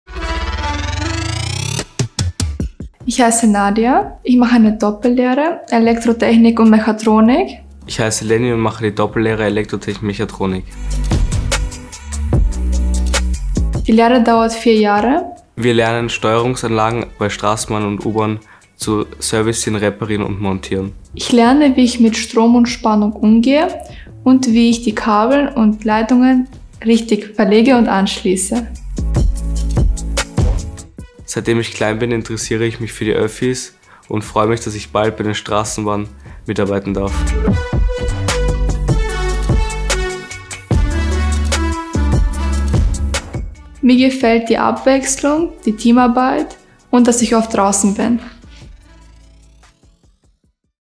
Doppellehre Elektrotechnik und Mechatronik: Deine Lehre in der Wiener Stadtwerke-Gruppe
Doppellehre = doppelt Ehre! Lerne gleich zwei Trendberufe auf einen Schlag: Mit der Doppellehre Elektrotechnik und Mechatronik läuft deine Karriere vollautomatisch. Denn in dieser Ausbildung erfährst du alles, was mit der Erzeugung, Verteilung und Anwendung von elektrischer Energie zu tun hat. Du lernst aber auch, wie man Schaltpläne liest und tauchst tief ins Innenleben mechatronischer Systeme ein.